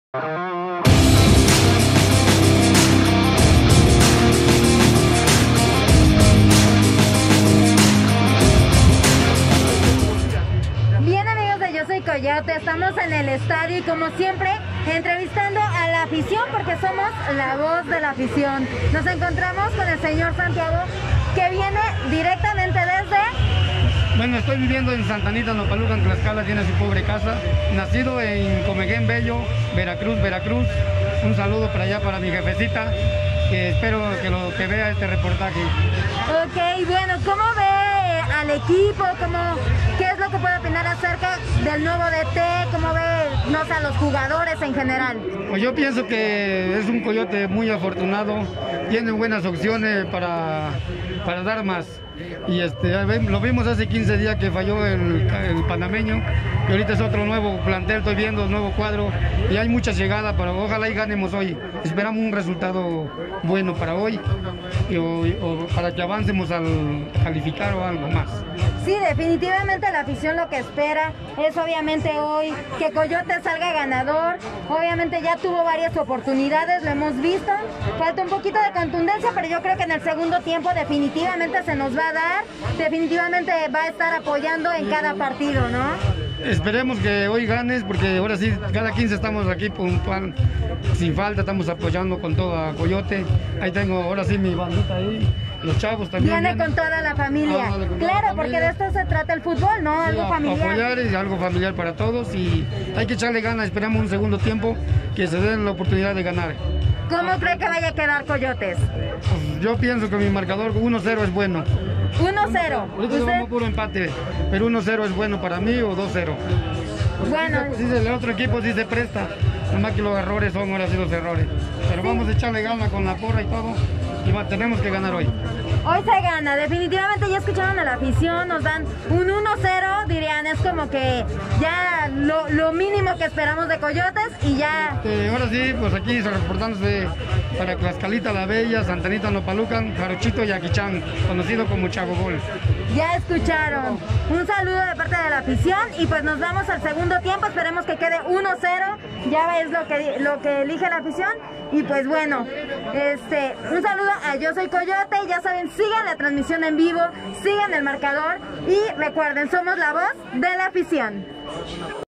LA AFICIÓN
El jugador numero 12 es la afición quien cada 15 días esta presente en el estadio y apoyando en todo momento a la jauría-